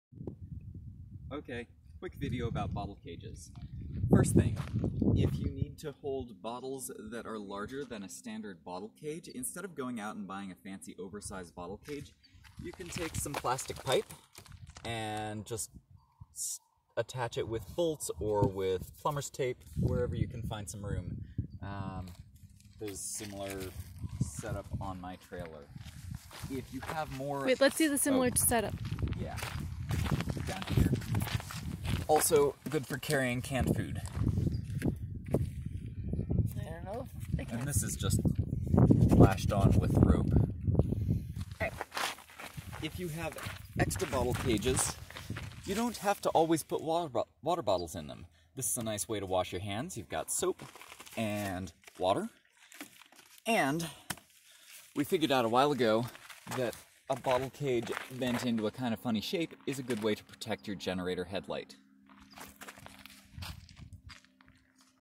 On Bottle Cages: Unintended Usage and Alternative Devices
Stopped at a rest area with a beautiful view and decided to do a few quick videos.  (With some major Helmet Hair!)

In this video:
- Use plastic pipes as cheap/free bottle cages (or to store canned food!)
- Store other things in your bottle cages
- Protect your recumbent's headlight with a bottle cage